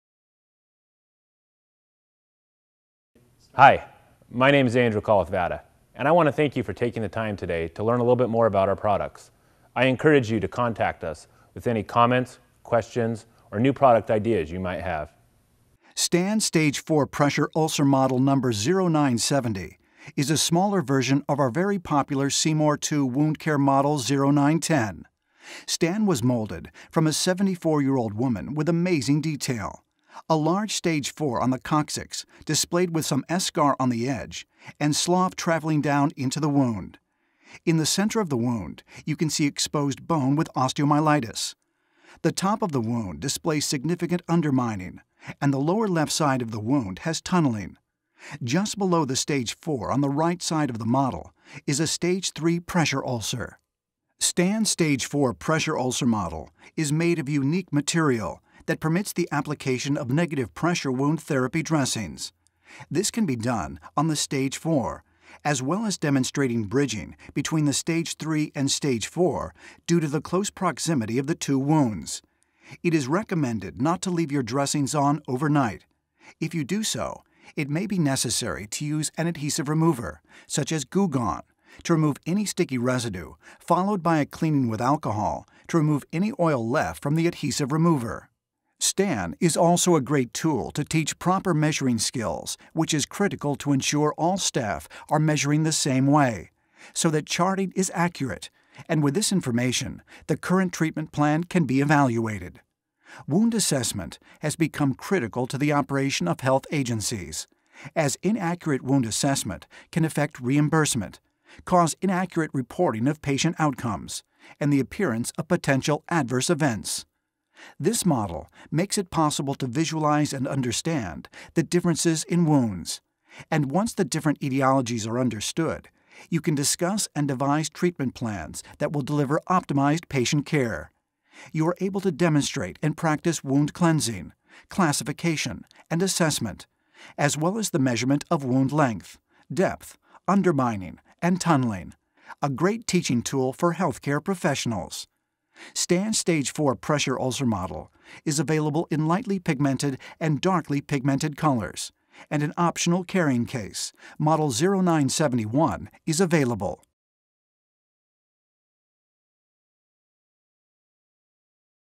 'Stan' Stage IV Pressure Ulcer Model™
This model features a large sacral Stage IV pressure ulcer, with eschar, subcutaneous fat, undermining, tunneling, slough, eschar and exposed bone with osteomyelitis and a Stage III pressure ulcer with subcutaneous fat and granulation tissue.